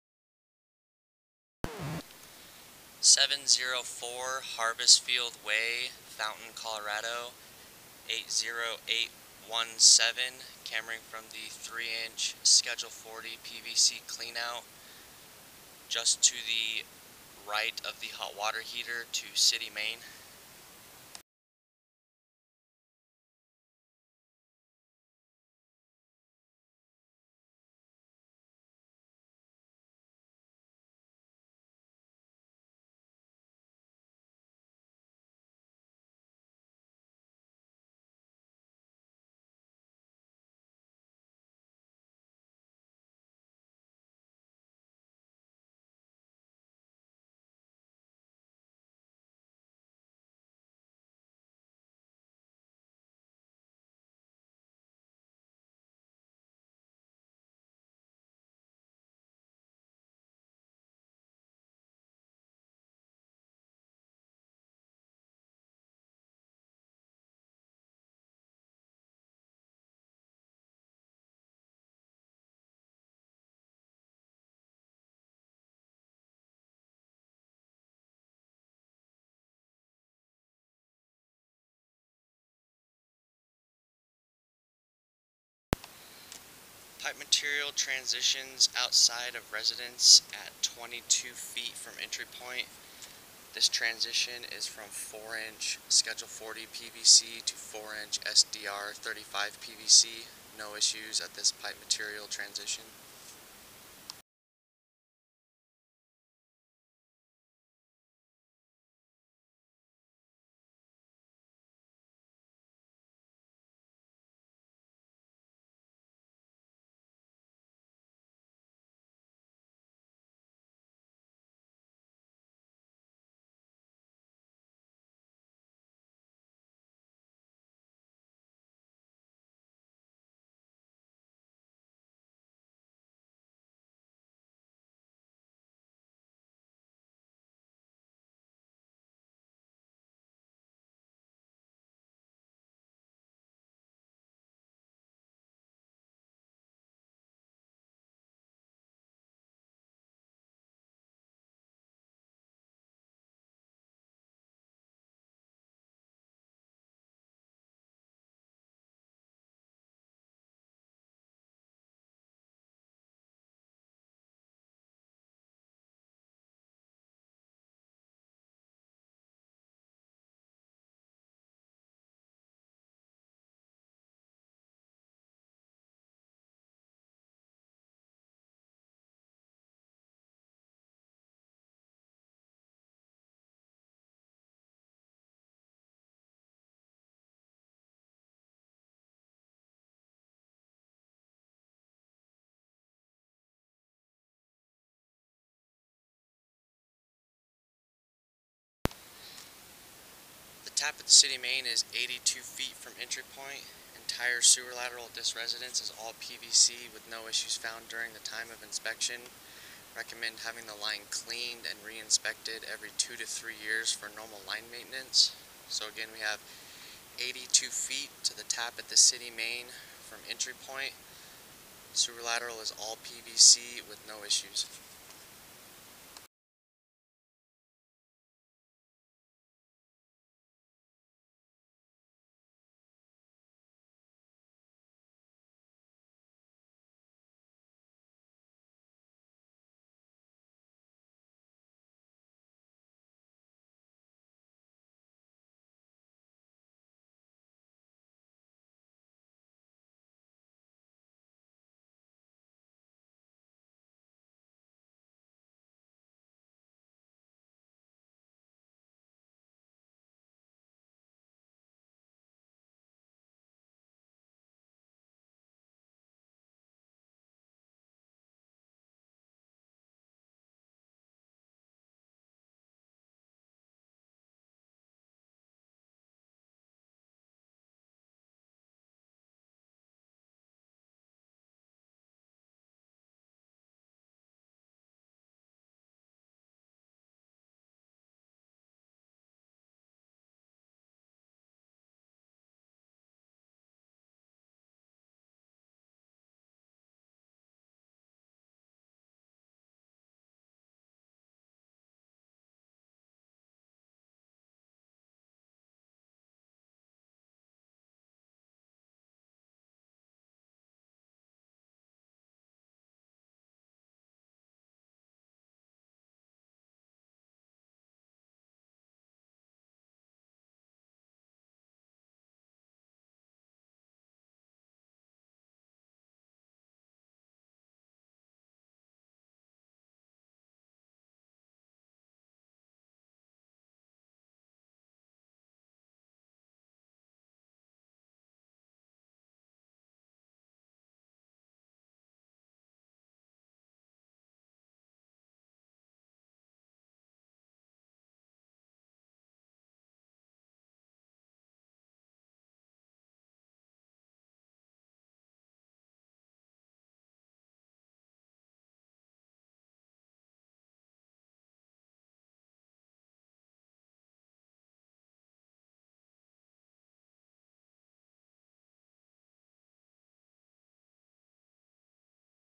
SEWER SCOPE 7/03/2023 704 Harvest Field Way Fountain, CO 80817
REPORT DETAILS:
ENTRY POINT:  SIZE, MATERIAL & LOCATION
- 3 in. SCH 40 PVC clean-out in basement to the right of the hot water heater

EXTERIOR TRANSITION: DISTANCE SIZE & MATERIAL
- 22 ft.
- 4 in. SCH 40 PVC to 4 in. SDR-35 PVC

ALL TRANSITION (S) W/ FOOTAGES:
- 00-22 ft. SCH 40 PVC
- 22-82 ft. SDR-35 PVC

DISTANCE TO THE TAP AT THE CITY MAIN
- 82 ft.

SIZE AND MATERIAL OF THE TAP AT THE CITY MAIN
- 4 in. SDR-35 PVC

ISSUES, RECOMMENDATIONS & OR OTHER DETAILS
- The sewer lateral at the residence is all PVC and passes all inspection requirements with no issues found at the time of the inspection. The sewer lateral at this residence demonstrates good flow all the way to the tap at the city main. Everything has been properly plumbed and glued with no joint separations, damages or major bellies found during the time of the inspection. We recommend having the line cleaned and re-inspected every 2-3 years for normal line maintenance. Only flush toilet paper. Do not put any wet wipes, “flushable we wipes”, grease or oil down any drains. If you have garbage disposal, only use it if necessary for bio-degradable food products.